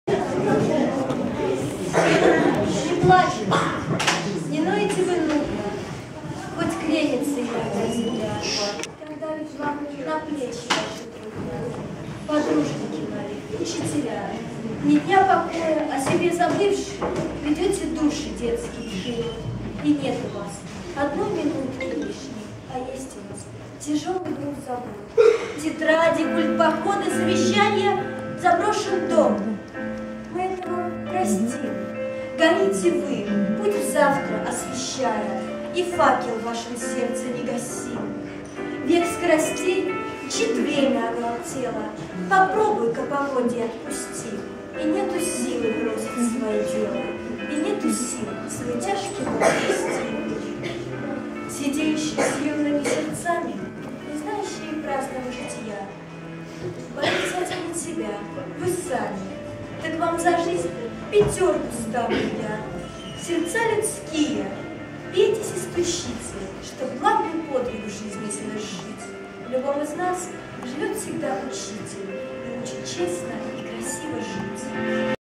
Декламирование стихотворения, посвященного Дню Учителя
Ученица 8 "А" класса